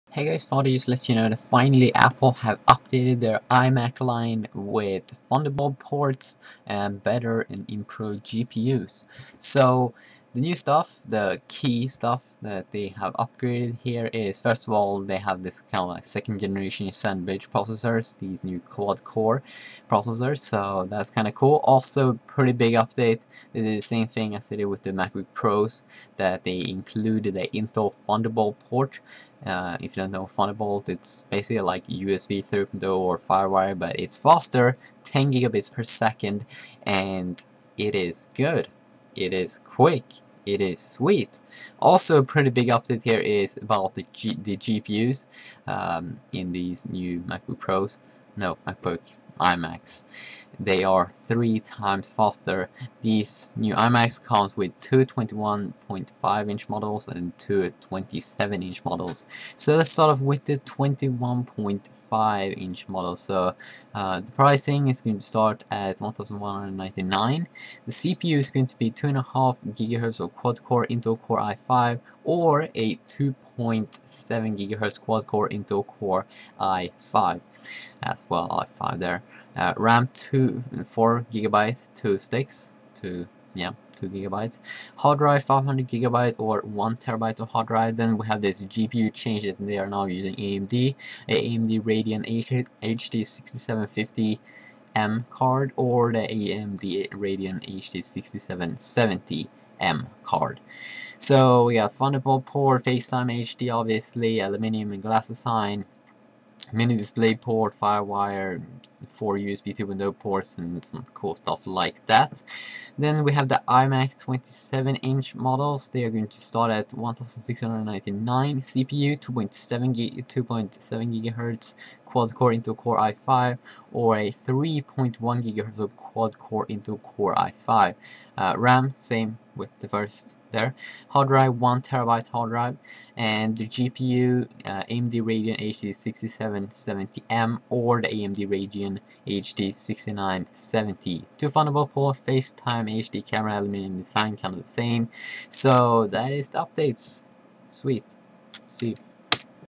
iMacs 2011 Lineup! New ThunderBolt Port & 3x Faster GPU! AMD Radeon HD 6750/6770/6790!
The new apple iMac computers is here! Specs are:

iMac 21.5"

Price: Starting at US$1,199
CPU: 2.5 GHz quad-core Intel Core i5 or 2.7 GHz quad-core Intel Core i5
RAM: 4GB (2 x 2 GB)
HDD: 500 GB or 1 TB hard drive
GPU: AMD Radeon HD 6750M or AMD Radeon HD 6770M
Other: Single Thunderbolt port, FaceTime HD camera, aluminum and glass design, IPS LED-backlit high resolution display, Mini DisplayPort, One FireWire 800 port, Four USB 2.0 ports, SDXC card slot, Slot-loading 8x SuperDrive, Audio in/out, Gigabit Ethernet, IR receiver, 802.11 a/b/g n Wi-Fi, Bluetooth 2.1 + EDR and an Apple wireless keyboard with a Magic Mouse or Magic Trackpad.
iMac 27"

Price: Starting at $1,699
CPU: 2.7 GHz quad-core Intel Core i5 or 3.1 GHz quad-core Intel Core i5
HDD: 1 TB hard drive
GPU: AMD Radeon HD 6770M or AMD Radeon HD 6970M
Other: Two Thunderbolt ports, FaceTime HD camera, aluminum and glass design, IPS LED-backlit high resolution display,Mini DisplayPort, One FireWire 800 port, Four USB 2.0 ports, SDXC card slot, Slot-loading 8x SuperDrive, Audio in/out, Gigabit Ethernet, IR receiver, 802.11 a/b/g n Wi-Fi, Bluetooth 2.1 + EDR and an Apple wireless keyboard with a Magic Mouse or Magic Trackpad.